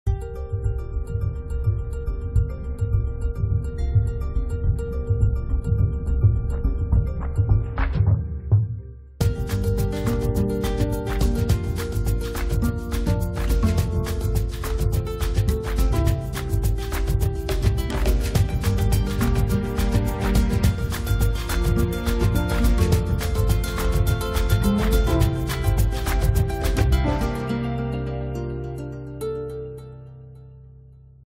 SM Men's Fashion Show at PFW Spring/Summer 2014
SM Men's Fashion Show at Philippine Fashion Week Spring Summer 2014 featuring British male supermodel David Gandy.